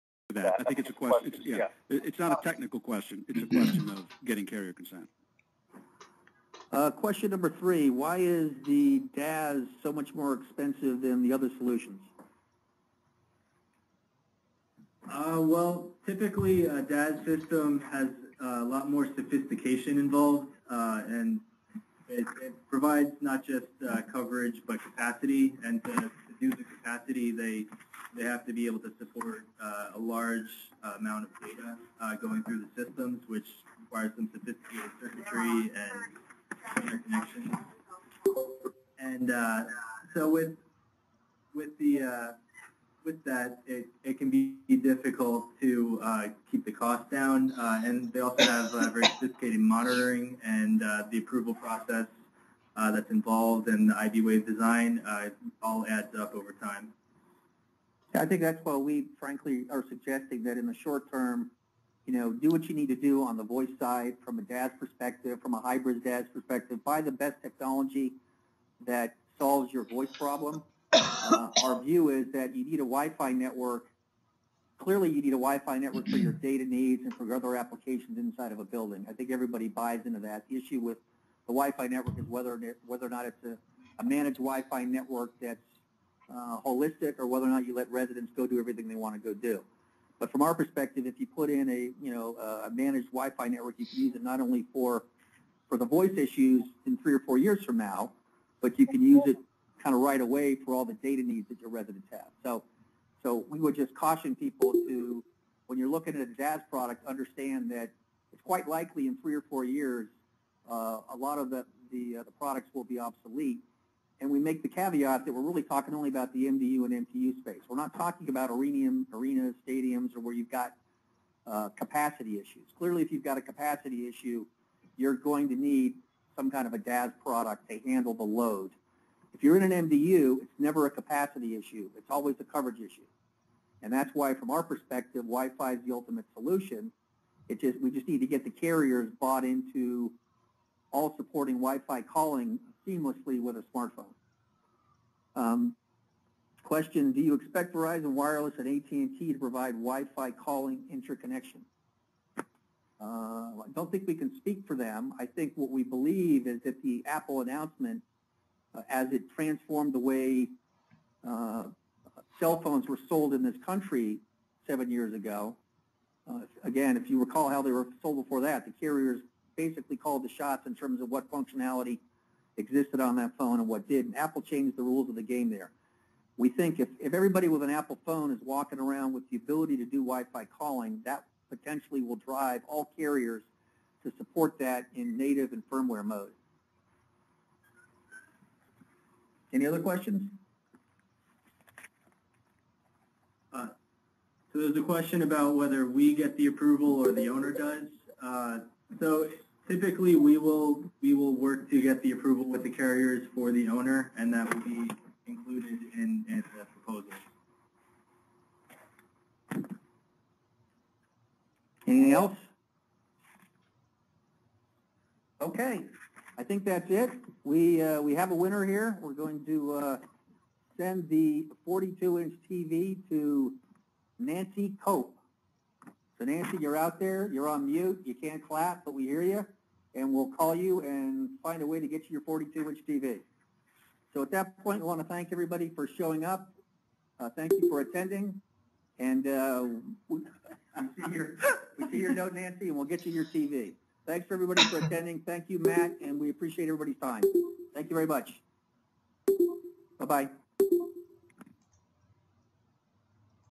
Part 5/5 - In-Building Cellular: What Building Owners Need To Know
DescripOn May 1st, 2014 a new FCC report and order went into effect that put regulations on the use of both consumer and industrial signal boosters.  The MDU/ MTU industry is wrought with in-door cellular problems - so which solutions work best for a property experiencing poor indoor cellular?  This webinar discusses which solutions work best for which properties touching on DAS, Small Cells, Consumer Boosters, CellBoost and WiFi Calling.
Matt Ames, attorney with Hubacher and Ames specializing in telecommunications law was the guest speaker.  Tom Doyle, President and COO of Spot On Networks presented.
tion